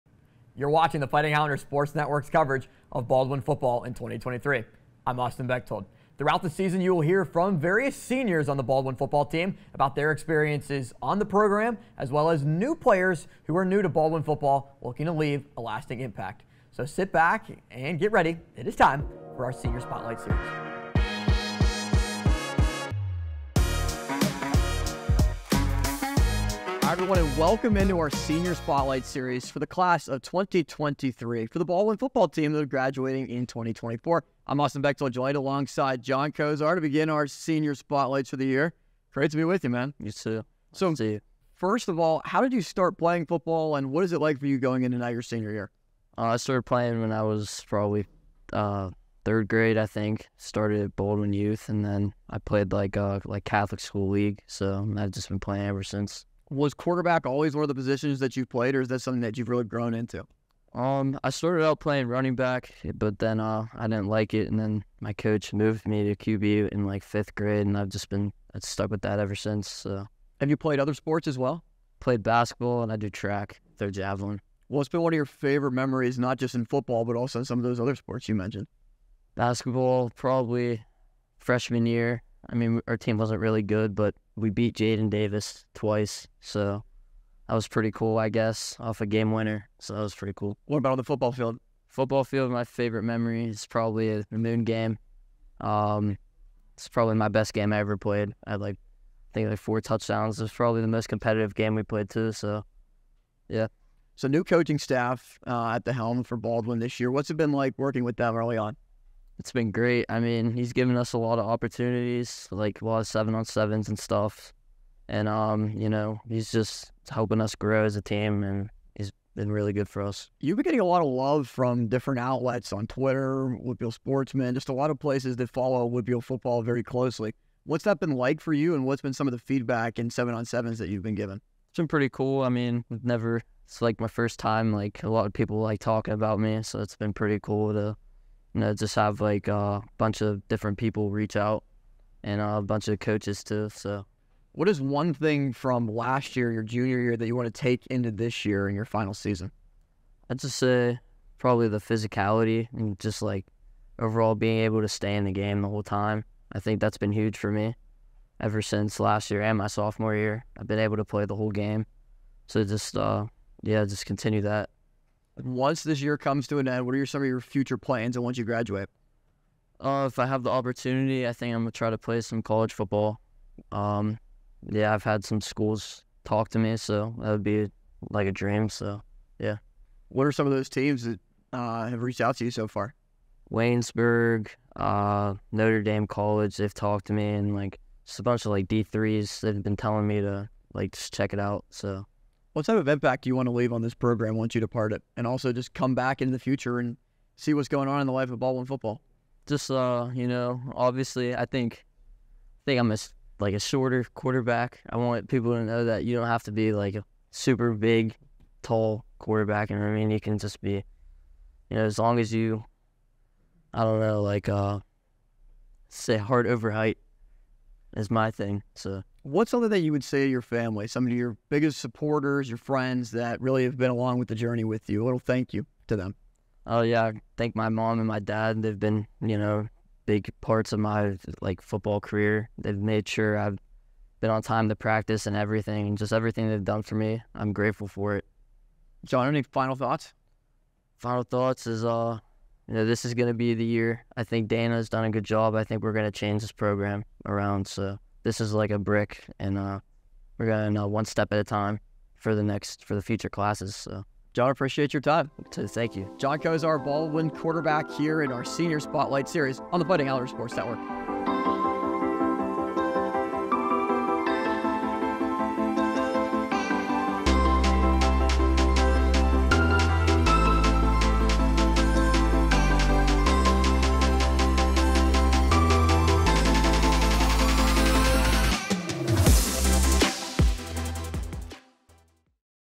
Senior Spotlight: John Kozar
Austin Bechtold sits down with senior John Kozar to discuss his thoughts on the Fighting Highlander Football season, his past experiences with the game, online recognitions, and his future beyond the Fighting Highlanders.